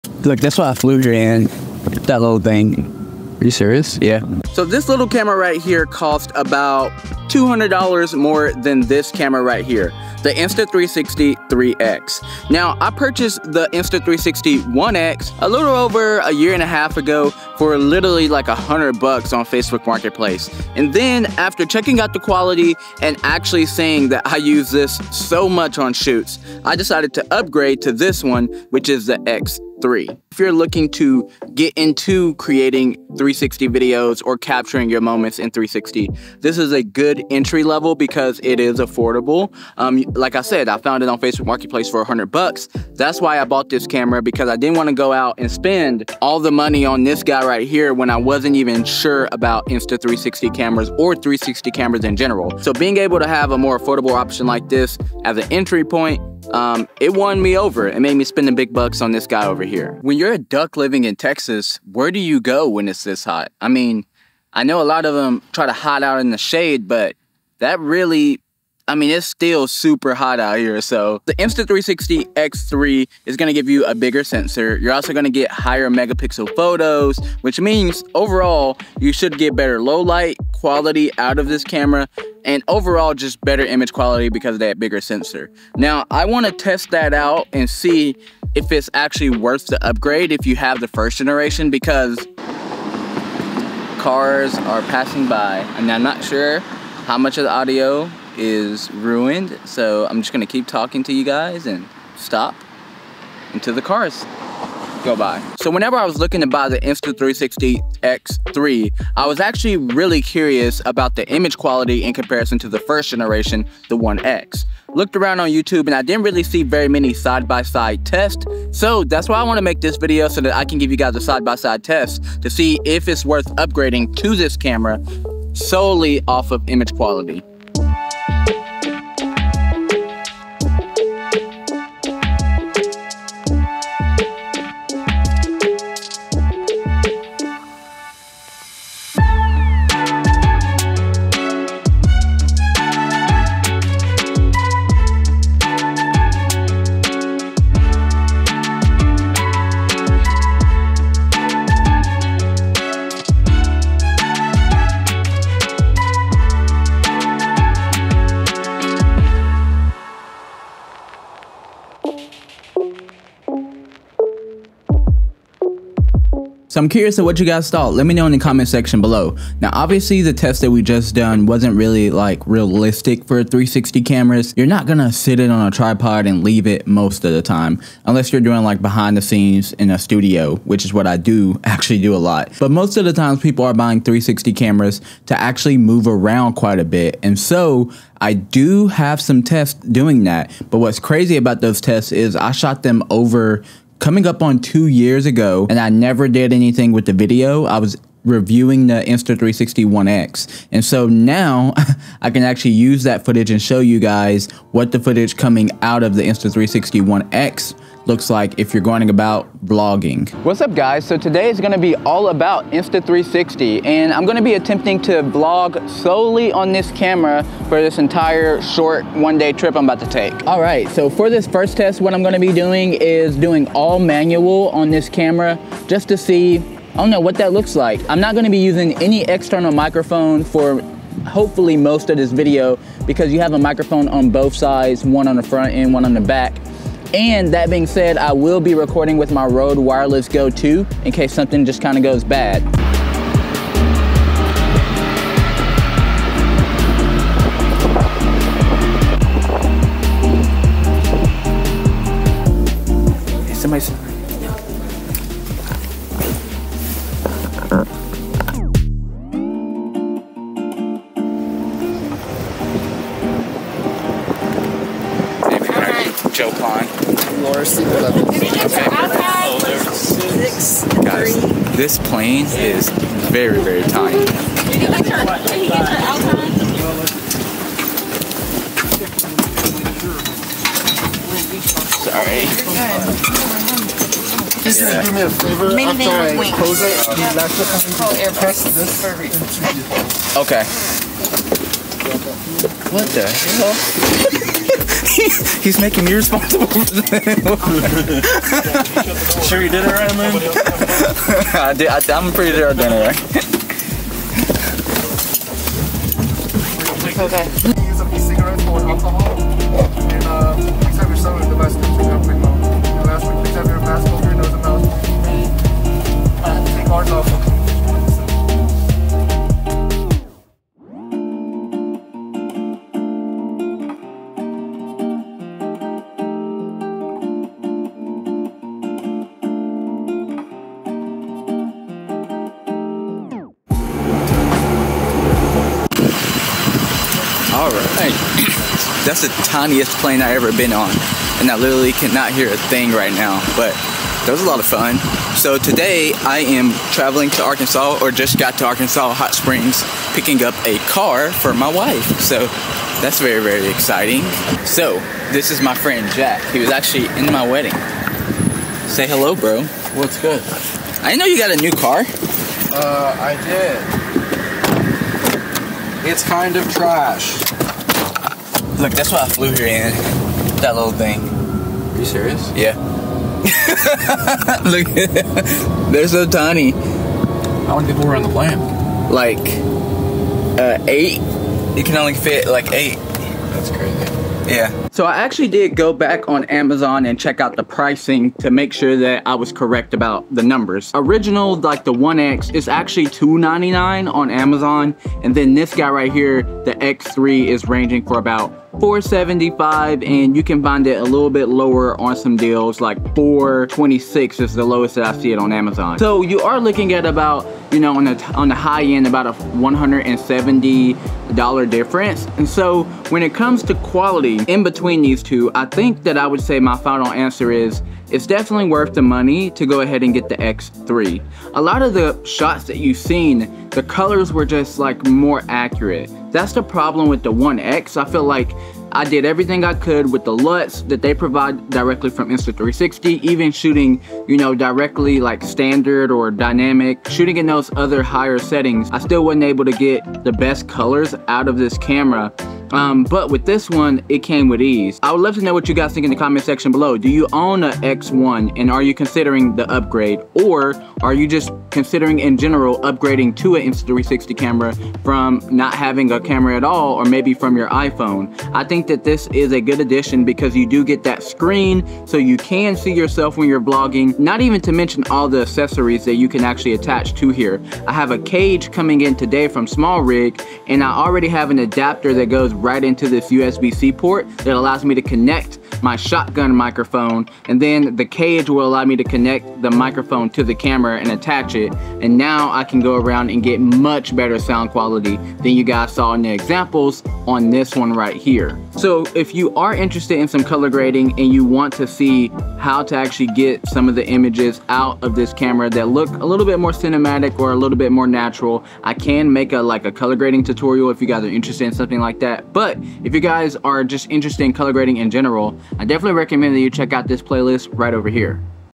Insta 360 X3 vs ONE X
After purchasing my first Insta360 camera, I naturally wanted to upgrade the quality. However, upon searching the Internet, I didn't find exactly what I was looking for in terms of side-by-side comparisons between the Insta360 One X and the Insta360 X3. Therefore, in this test, I aimed to provide a quality comparison so that others in my position could make the right choice.

For insights into the equipment employed in the production of this episode, please refer to the gear used to film this episode